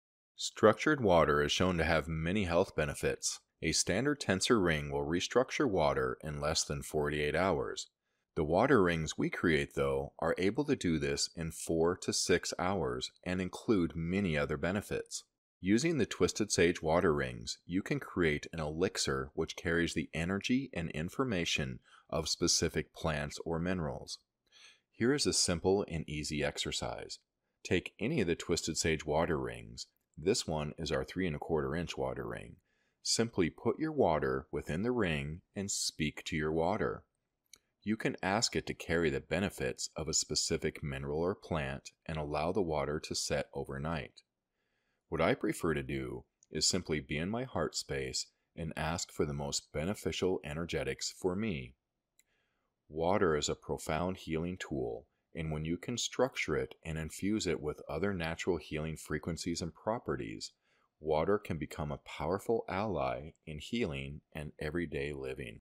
Creating Healing Elixirs with Water
Structured water is shown to have many health benefits.  A standard Tensor Ring will restructure water in less than 48 hours.   The Water Rings we create though, are able to do this in 4 to 6 hours, and include many other benefits.
Using the Twistedsage water rings, you can create an elixir which carries the energy and Information of specific plants or minerals.
Here is a simple and easy exercise.
Take any of the Twsitedsage water rings, this is our 3-1/4" Water Ring.  Simply put your water Within the Ring, and speak to your water. You can ask it to carry the benefits of a specific mineral or plant , and allow the water to set overnight.
What I prefer to do, is simply be in my heart space, and ask for the most beneficial energetics for me.
Water is a profound healing tool, and when you can structure it and infuse other natural healing frequencies and properties into it, water becomes a powerful ally in healing and everyday living.